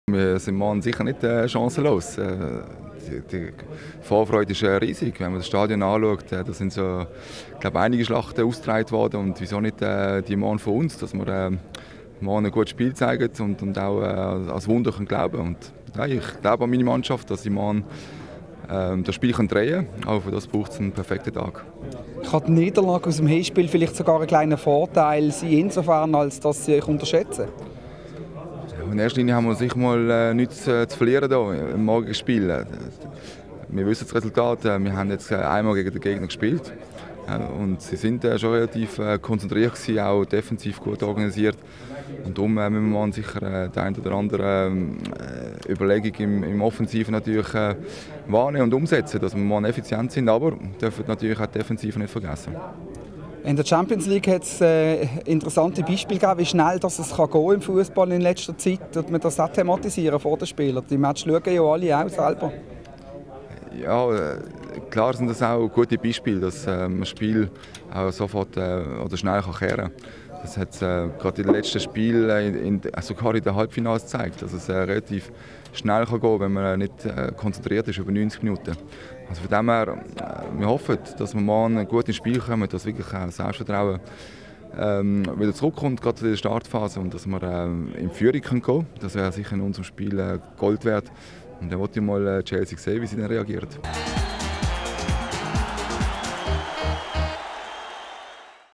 Interview mit Murat Yakin
Interview mit Murat Yakin vor Chelsea-FCB, 2. Mai 2013